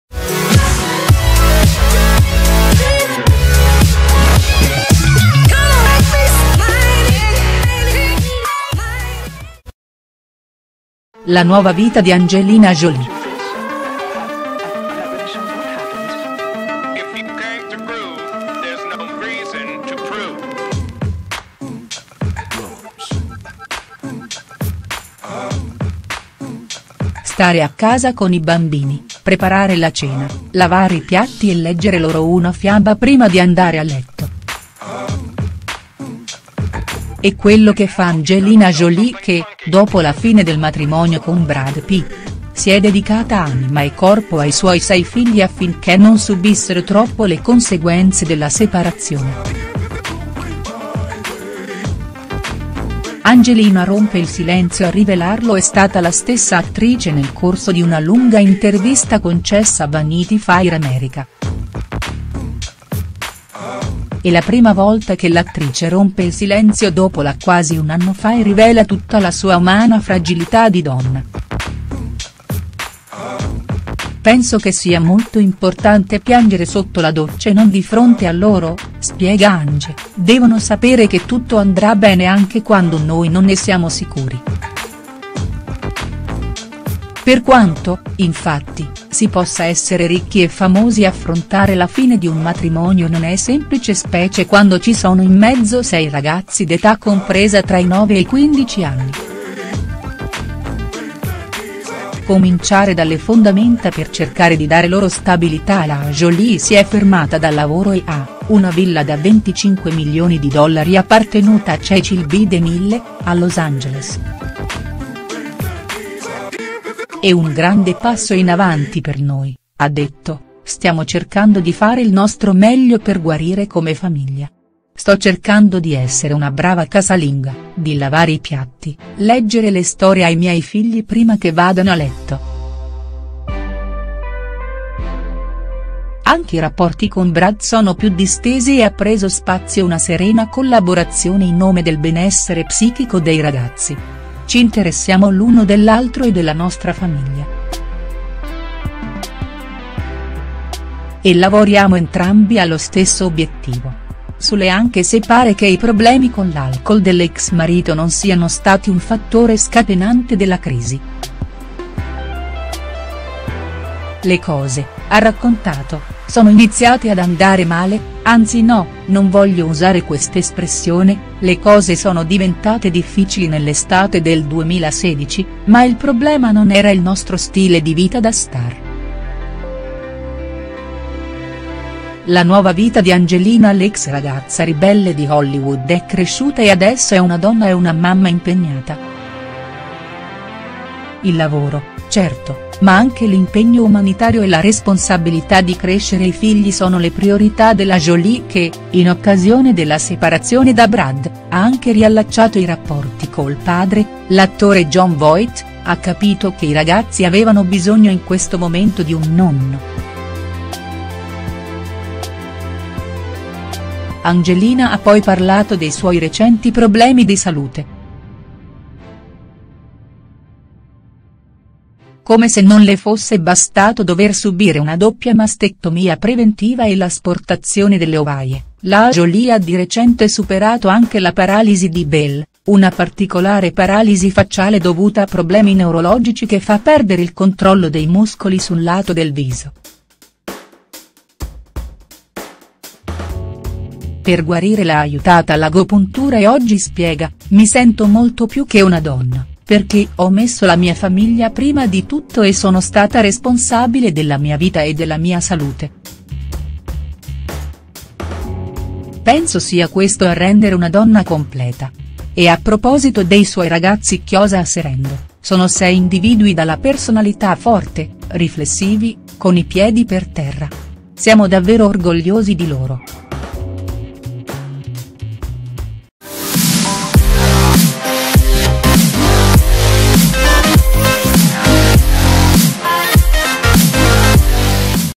La nuova vita di Angelina Jolie.HD
Per la prima volta dopo la fine del matrimonio con Brad Pitt l'attrice si racconta